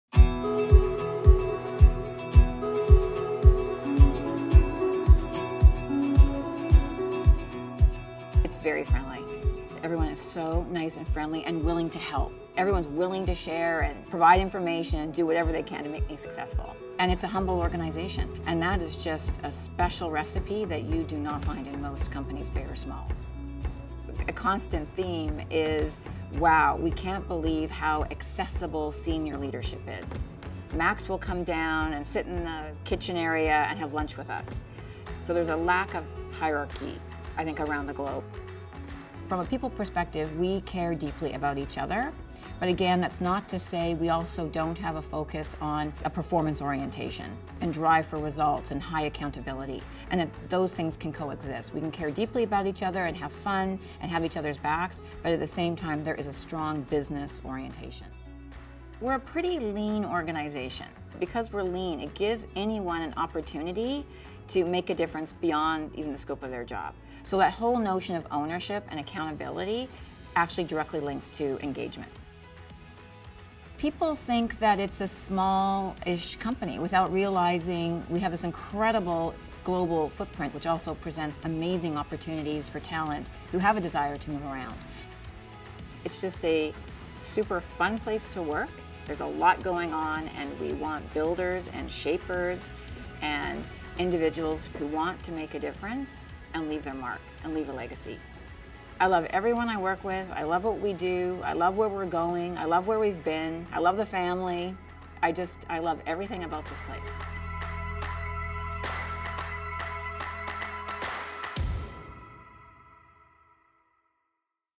The McCain Experience | Careers at McCain Foods
Alison DeMille, Chief Human Resources Officer at McCain Foods, shares her personal McCain Experience.